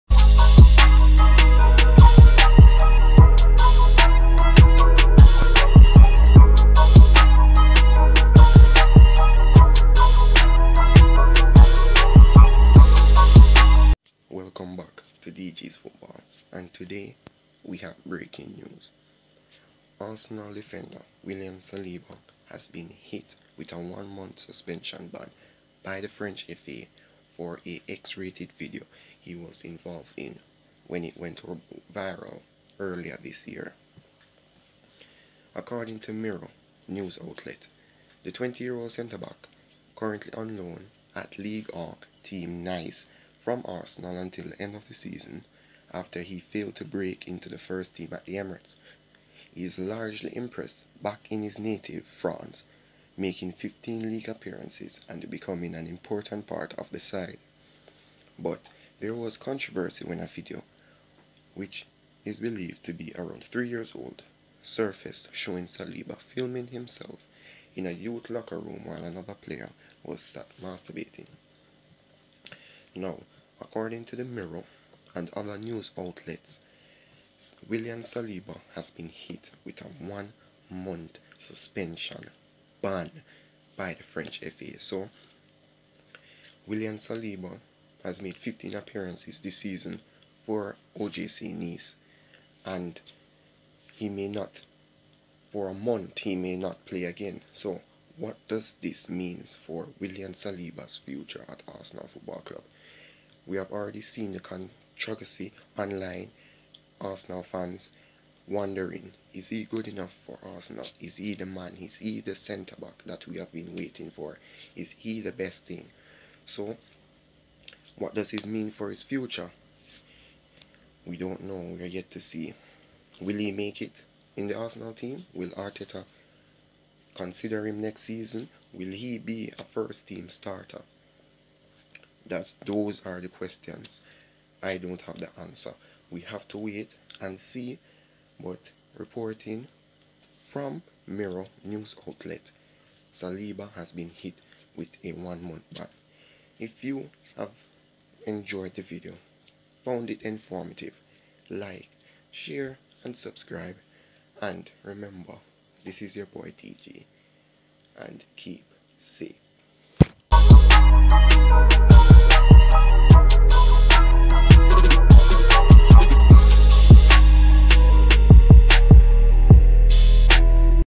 BRAKING 🚨| Saliba Has Been Banned By The French FA For Viral video 😮
Today's Video is a Braking News Update Arsenal Center Back Williams Saliba Has Been Banned By The French FA For X Rated video which went viral
What are your Thoughts do you Think Saliba was rightly Banned By The French FA ? Or You Think it's Harsh?